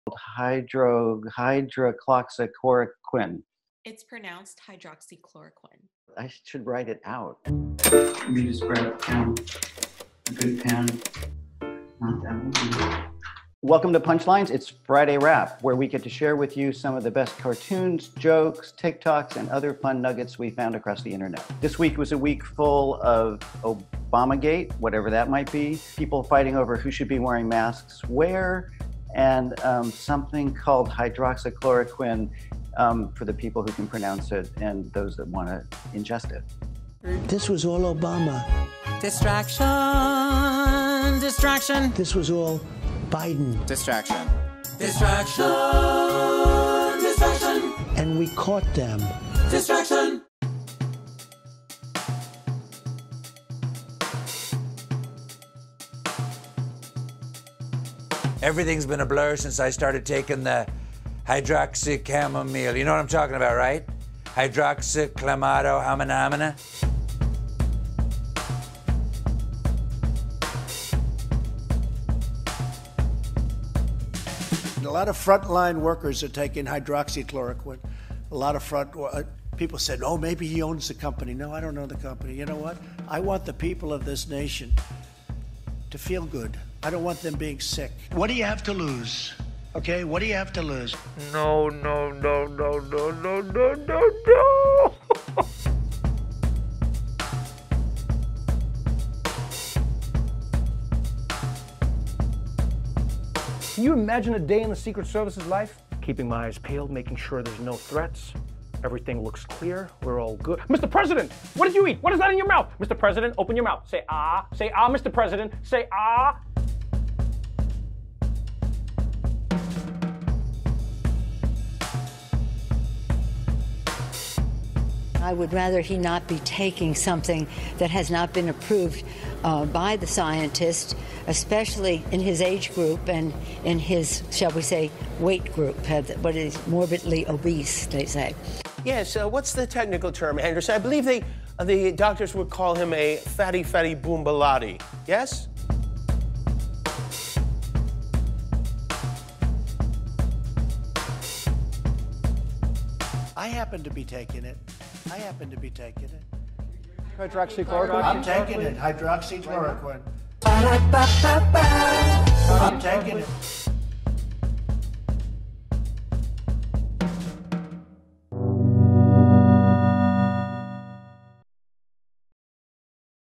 TikTokers, comedians and cartoonists roast Trump's use of hydroxychloroquine
Once again, the internet has broken over Trump's comments on using hyhydroxychloroquine and Obamagate. In this episode of PUNCHLINES, Matt Wuerker shares some of this week's political satire.